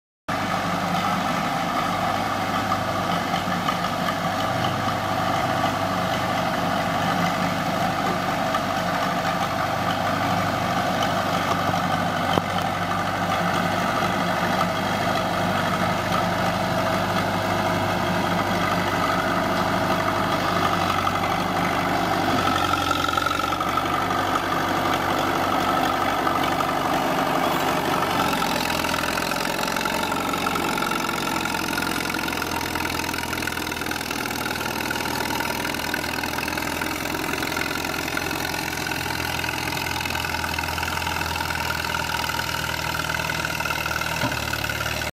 D4 with La Plante Choate CM41 scraper
deepening a path to put gravel in for an access road.